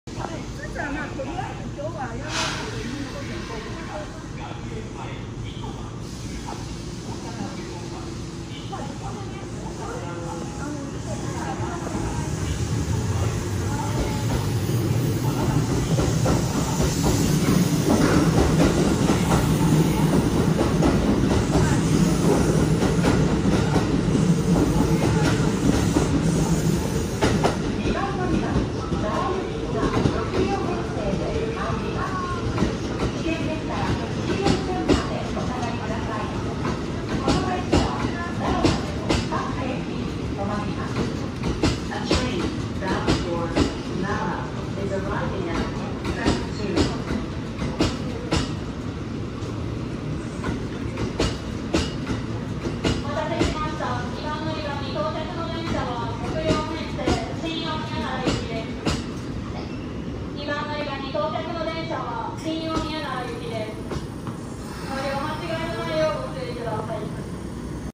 阪神なんば線へ直通！近鉄奈良線神戸三宮行き快速急行 大和西大寺到着
大和西大寺駅2番のりばに到着する近鉄奈良線5800系☆時52分発阪神なんば線へ直通する神戸三宮行き快速急行。